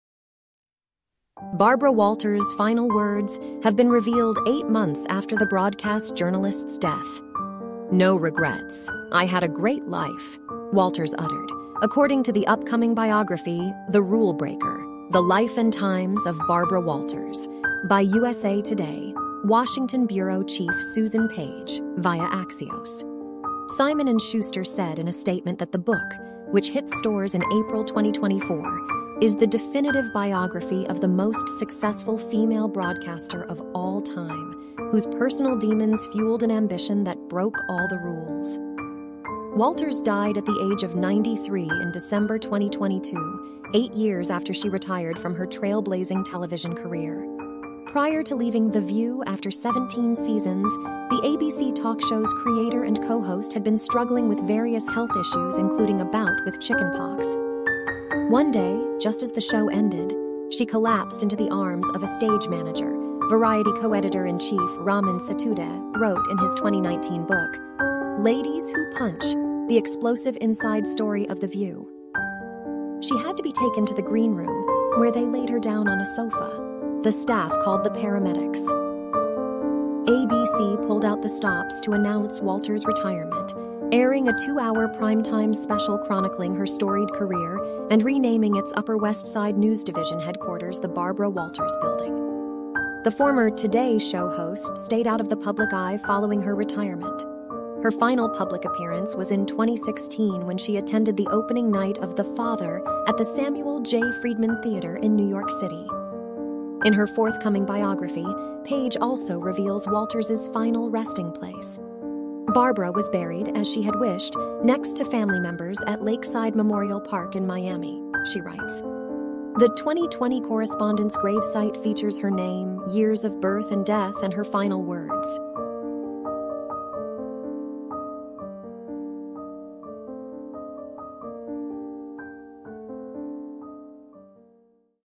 Barbara Walters’ final words revealed 8 months after her death : UTD News
Barbara Walters’ final words have been revealed eight months after the broadcast journalist’s death.

“No regrets — I had a great life,” Walters uttered, according to the upcoming biography “The Rulebreaker: The Life and Times of Barbara Walters” by USA Today Washington bureau chief Susan Page (via Axios).

Simon & Schuster said in a statement that the book, which hits stores in April 2024, is “the definitive biography of the most successful female broadcaster of all time … whose personal demons fueled an ambition that broke all the rules.”

Walters died at the age of 93 in December 2022, eight years after she retired from her trailblazing television career.

Prior to leaving “The View” after 17 seasons, the ABC talk show’s creator and co-host had been struggling with various health issues including a bout with chicken pox.